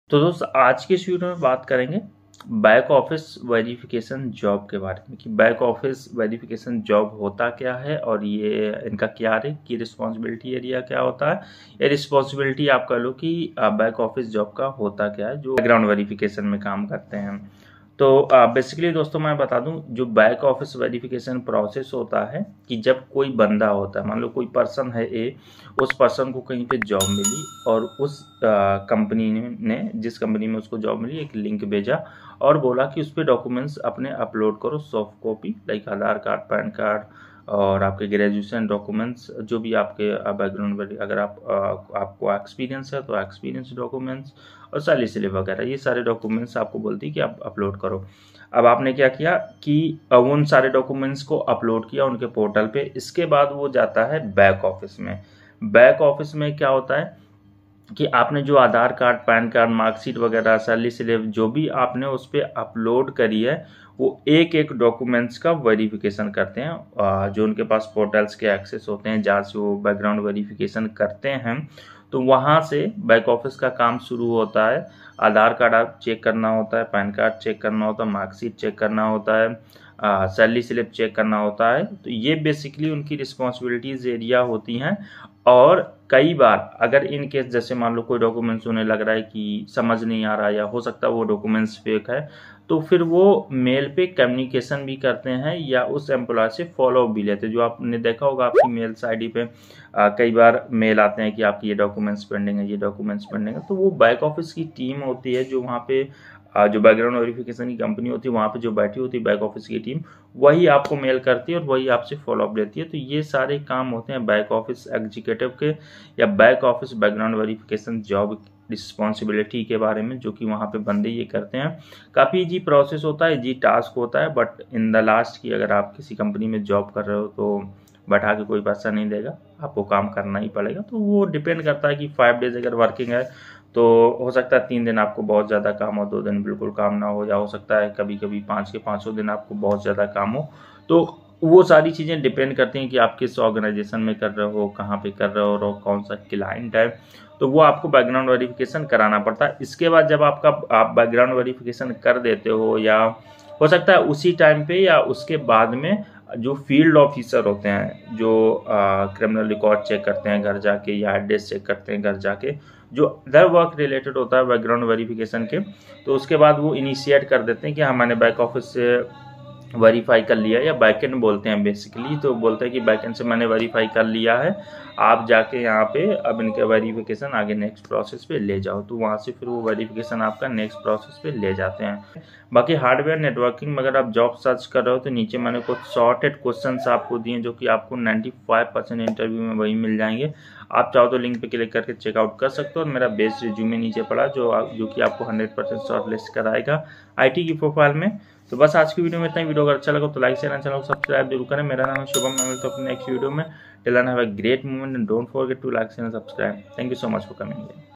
Back Office Verification Work | Get Job in background Verification Company | Private Verification
Maine is video me btaya h ki aapko back office verification ki job me kya kam krna padega or aapko ye job kaise milegi full detail me hindi me discuss kiya h i hope aapko video acha lage thanks

Thanks.